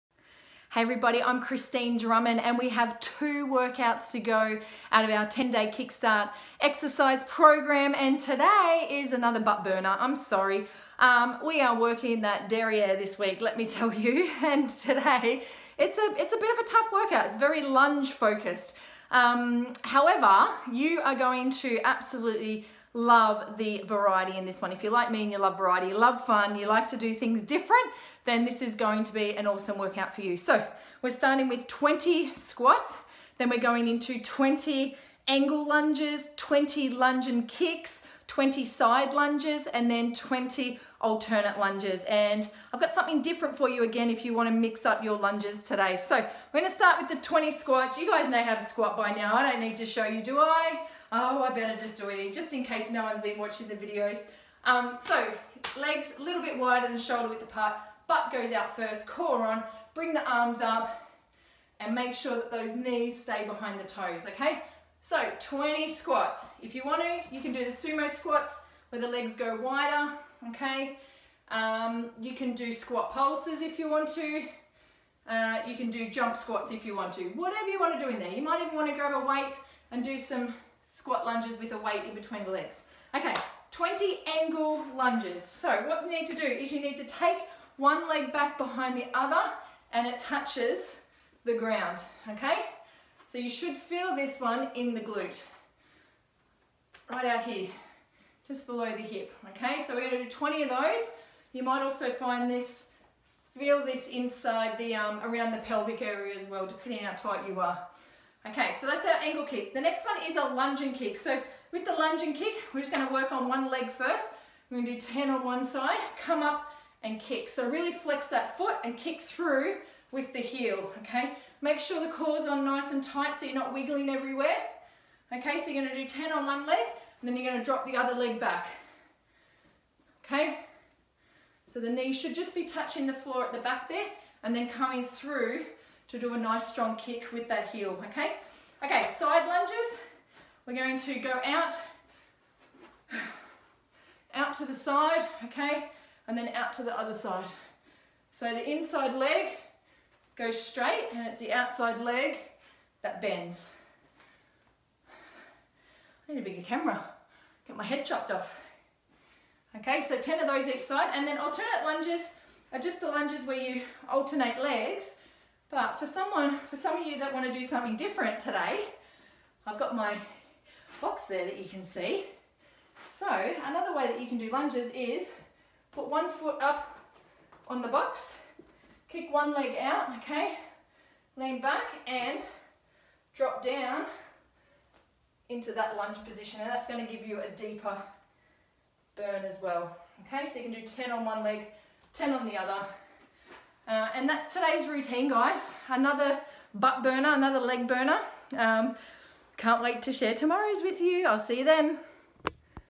Day 9 with Kristine Drummond | 10 Day Workout Routine
I think you’re going to love this one!!

One thing to remember is always warm up for 2-5mins first to get the synovial fluid flowing between the joints. Here are some warm up ideas:

Jogging in the spot
Walking
Jumping jacks
Bouncing on exercise tramp
Circling arms and leg motions
Standing high knees
Skipping
Riding

It’s a good idea to finish each workout with a few ab exercises to strengthen the core and also the back to reduce any pain and inflammation along the spine.

Most of all have fun with it and exercise at your own risk according to your own fitness level and injuries. If you are unsure if you should be performing exercise always consult a health care professional.